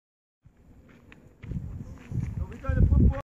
Ready Stance
Those who sought Zion during Sakuth 2015. The men ready to pray before the house and the women getting ready to WAIL!